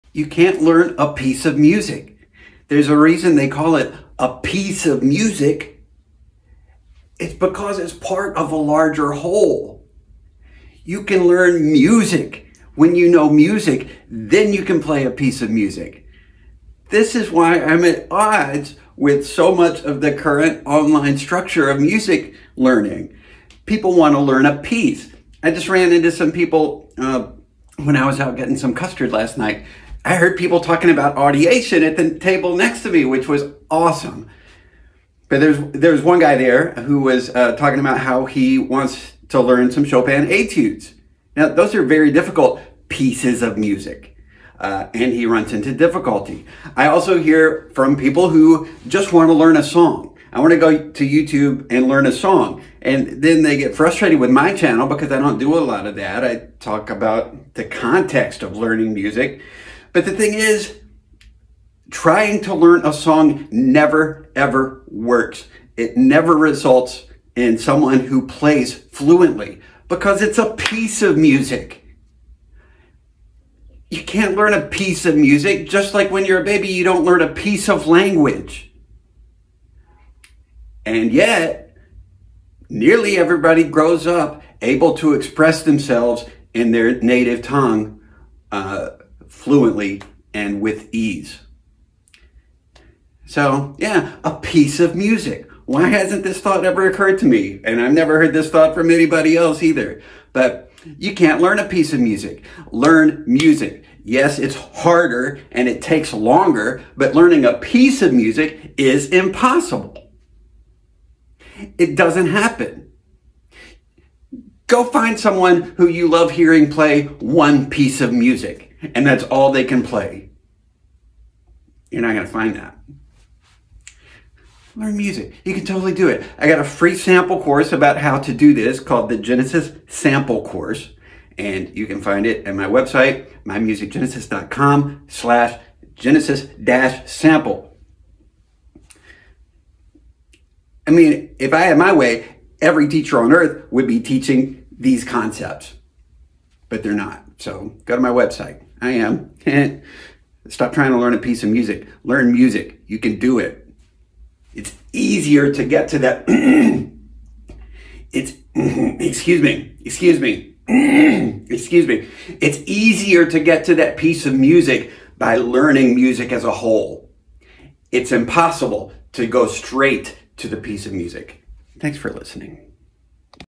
You will never learn a piece of music
Why the pantheon of former music students is filled with failure and what you can do to change that story.

Please forgive the audio. I had to get this idea out and forgot to mic up.

If you'd like to try my Genesis Sample Course you can do so

You'll just need to create an account with email, username, and password.

You can bypass the talky bits if you want but if you do you'll probably bypass my points.